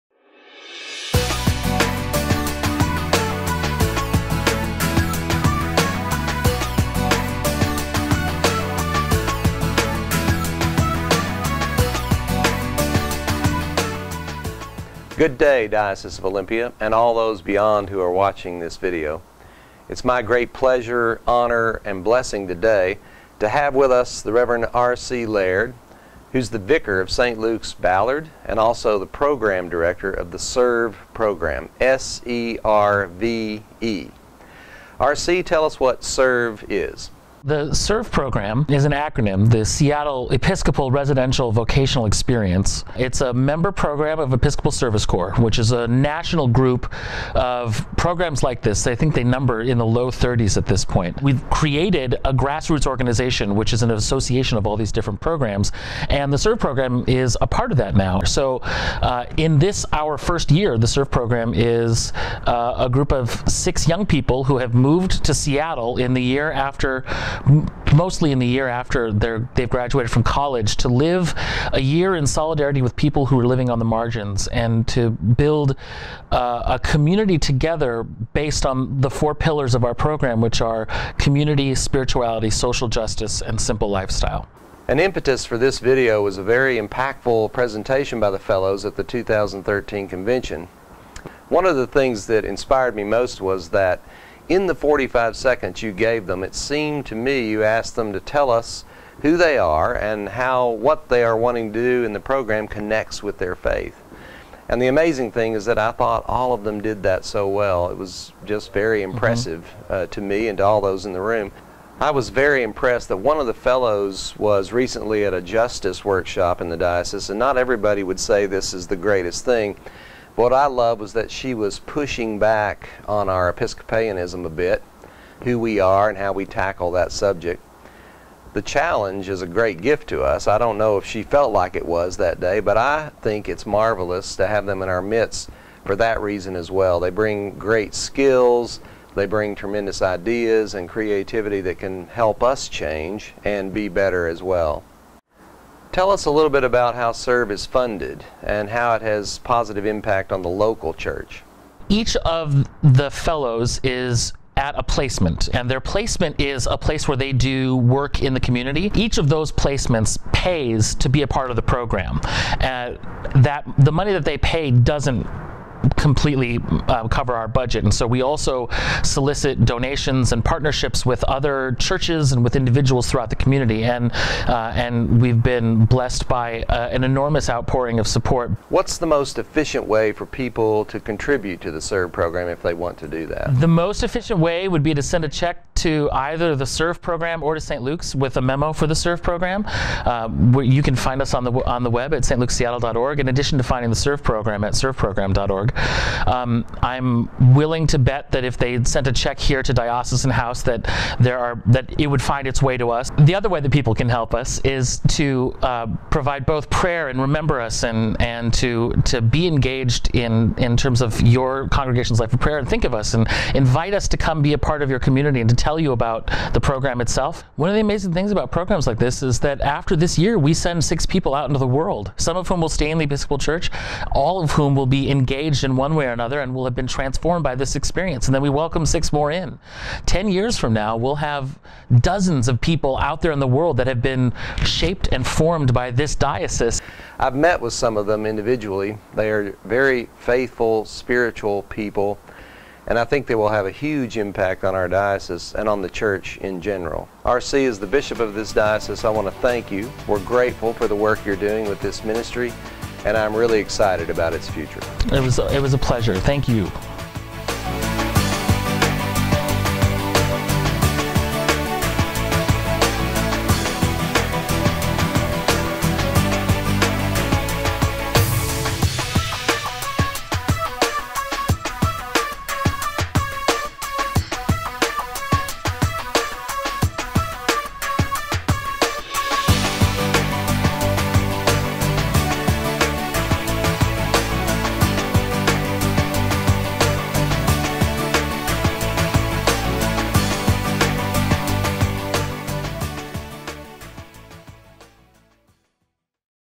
SERVE June 2014
A brief overview of the SERVE program in the Diocese of Olympia.